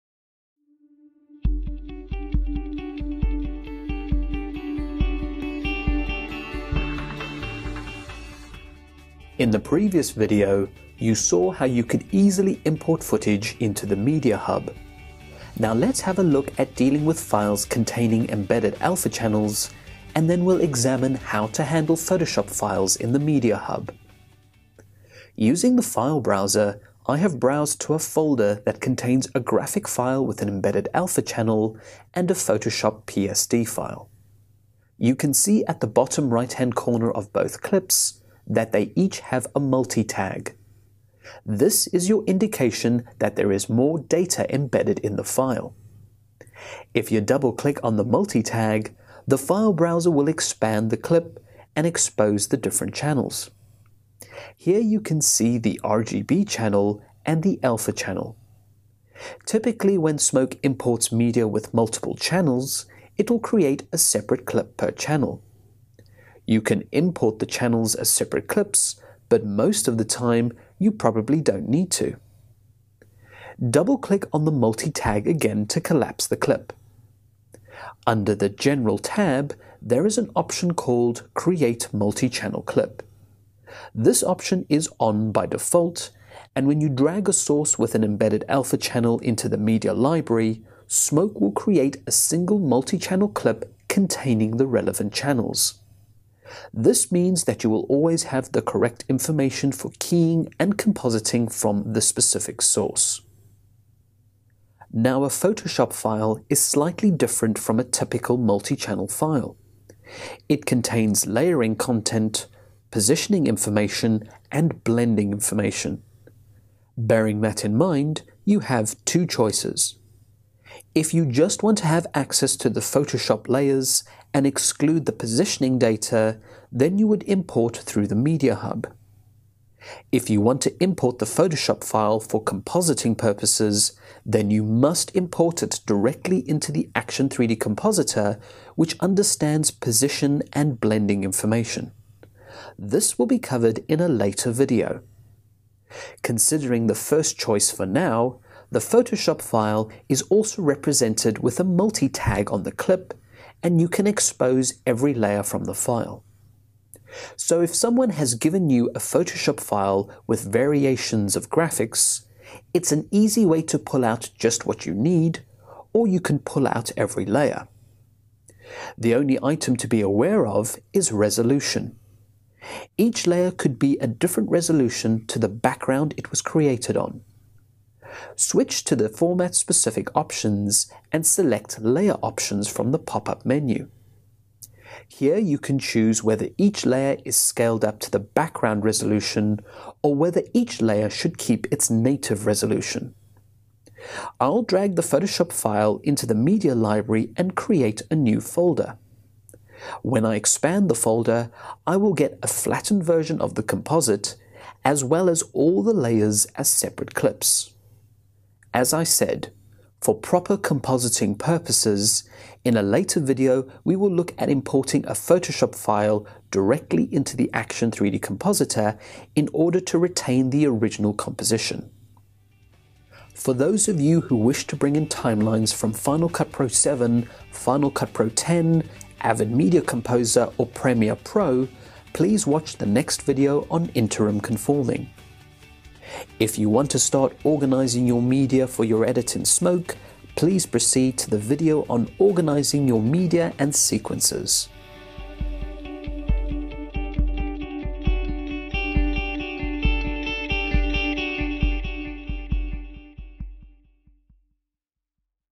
MediaHub: Dealing with Alpha and Photoshop PSD
Level: Beginner

This movie show you how to deal with files with an alpha as well as Photoshop PSD files in Autodesk Smoke.

For Information, Feedback and Comments:

Autodesk Area